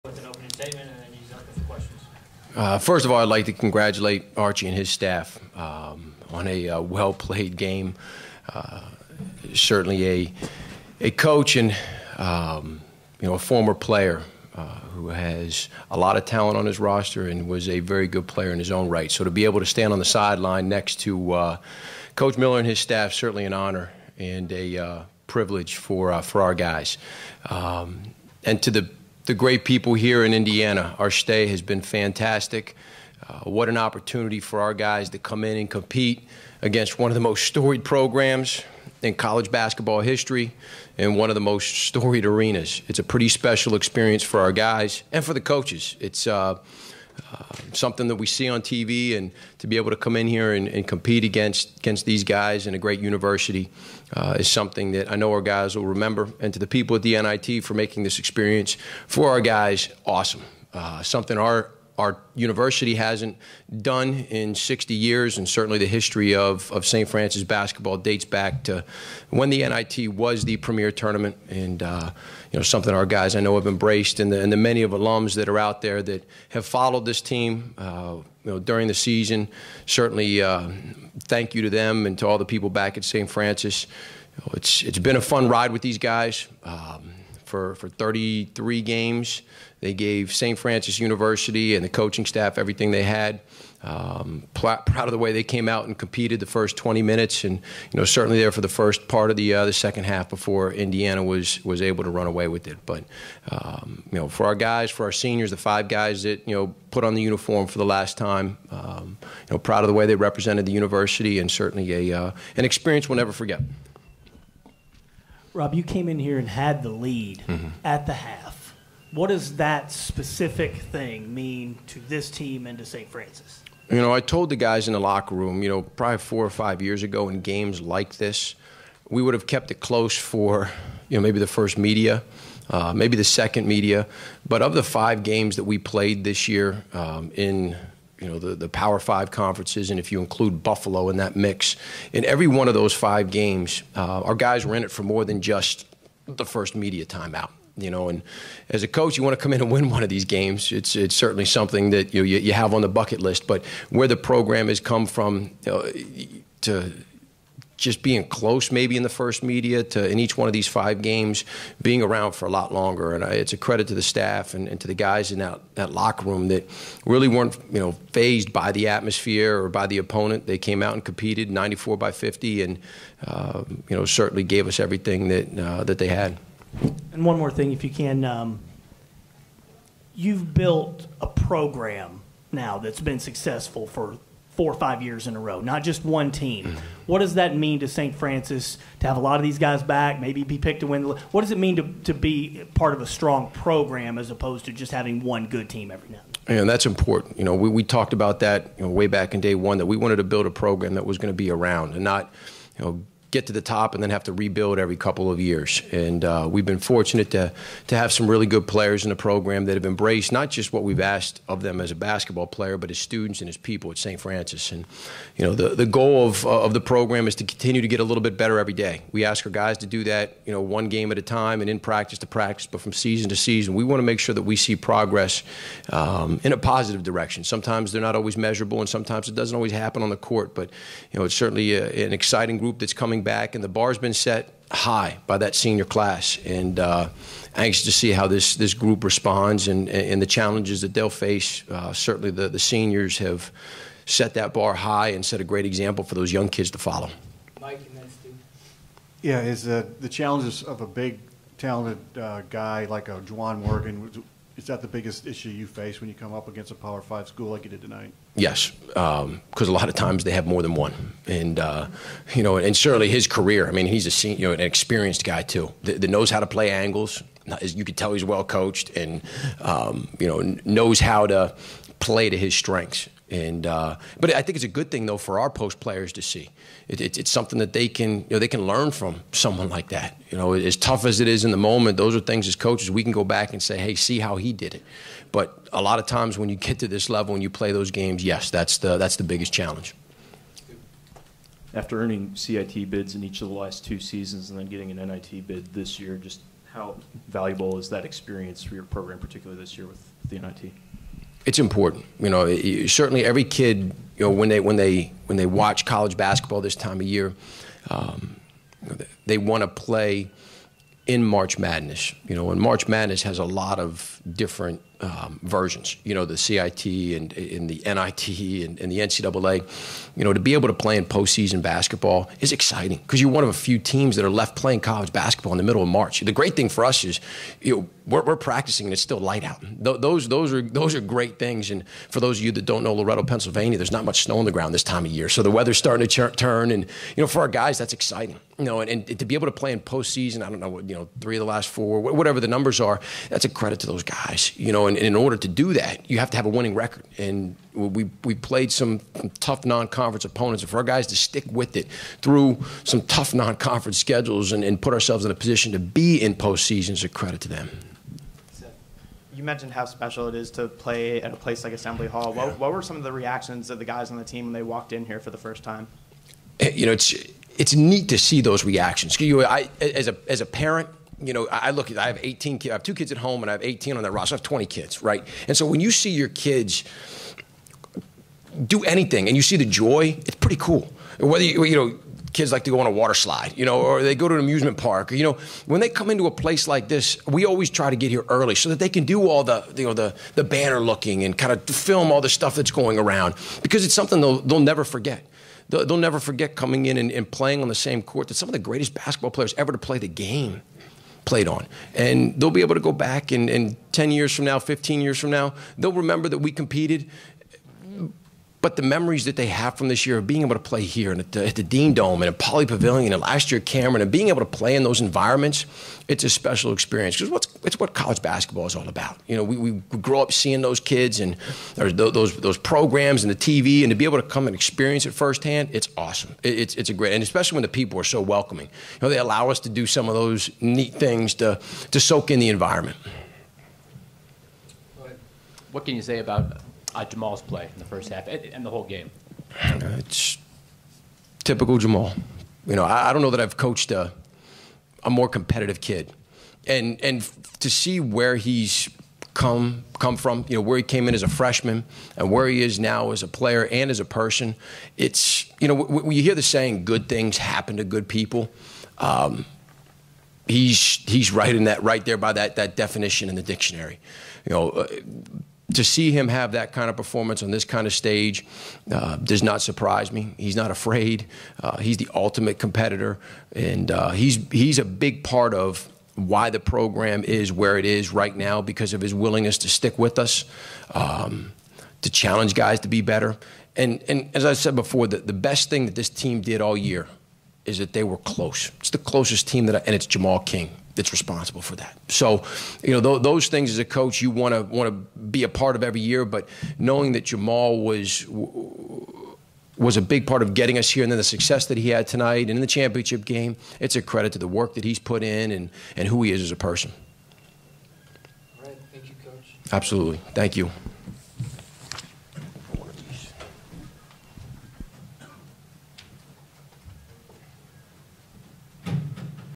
Video: Rival Reaction -- St. Francis (PA)
Watch and listen to what St. Francis (PA) head coach Rob Krimmel had to say following the Red Flash's loss to IU.